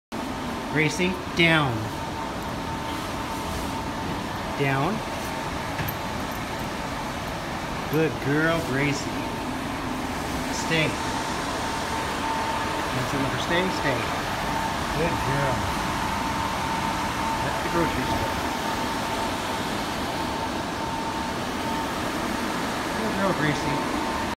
May 10, 2019
“Gracie”, 5 month old Maltese Puppy. Training & Socializing at Ralph’s grocery store.